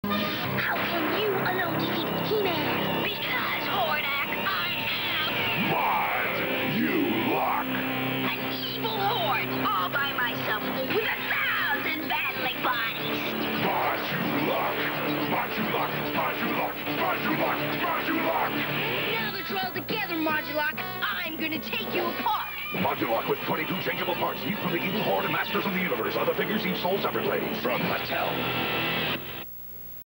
80's He-Man Toy Commercial Modulok
80'c commercial for the Masters of the Universe figure Modulok.